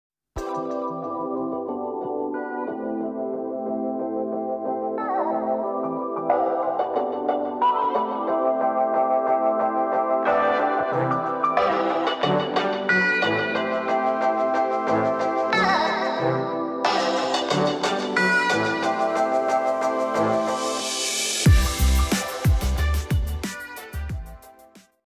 Экопринт на акварельной бумаге
Последовательная раскладка листьев на акварельной бумаге.